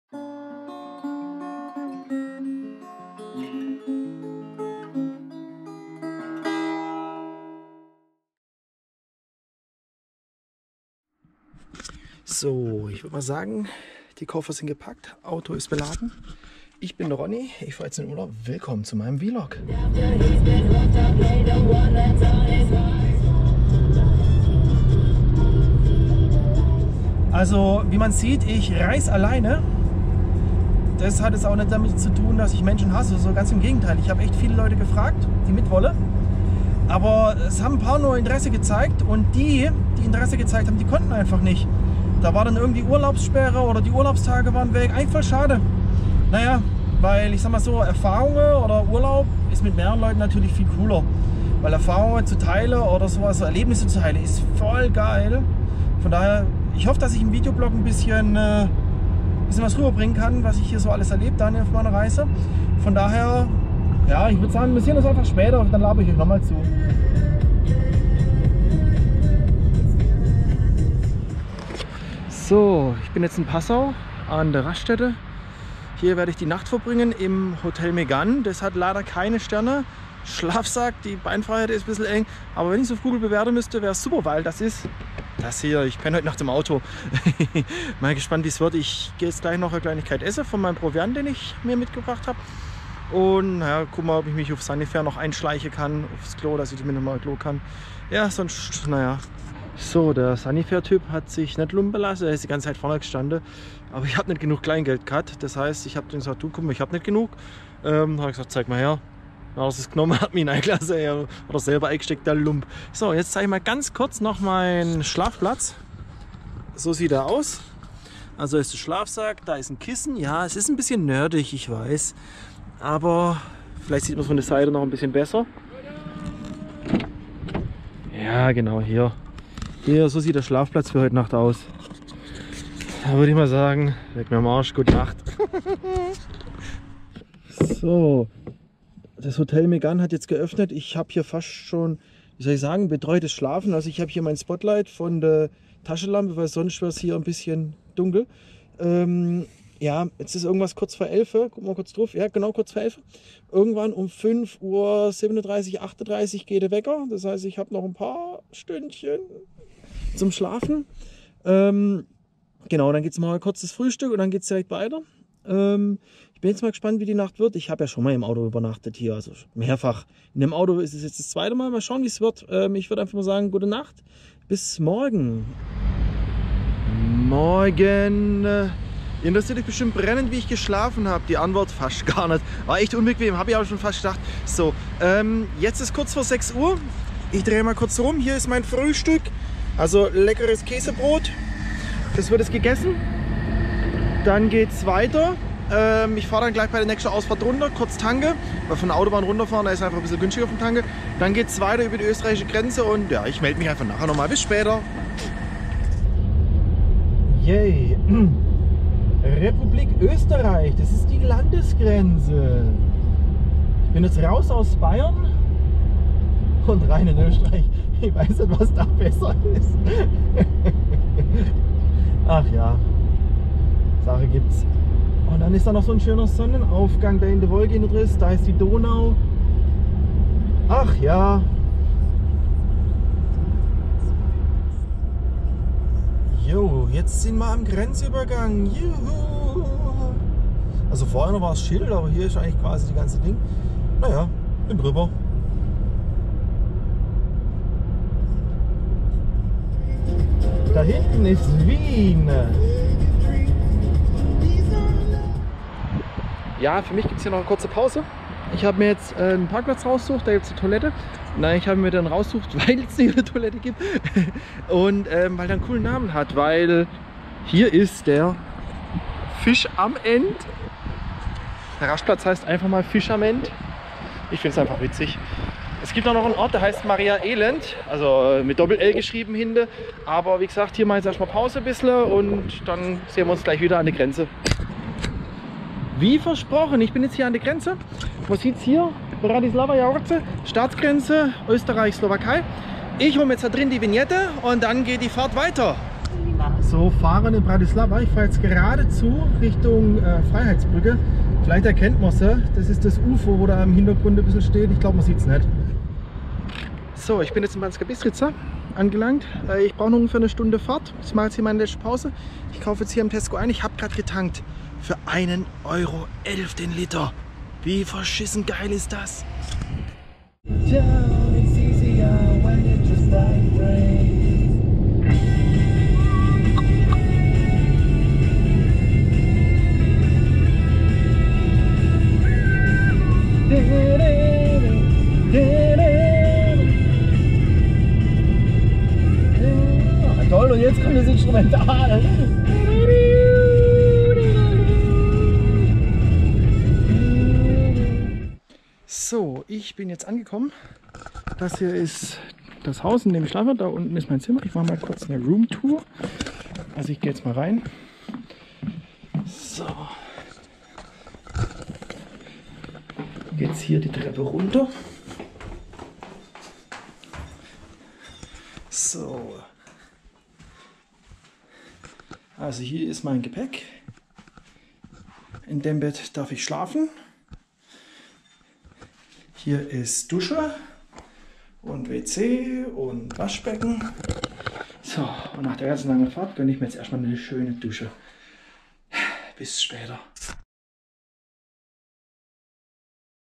slowakei urlaub - die anreise - vlog mit ronny #1
mein sommerurlaub im august 2020 in der slowakei muss ja auch mal anfangen.
Ich bin in der Gegend von Liptovsky Mikulas unterwegs.
und ja, ich reise alleine.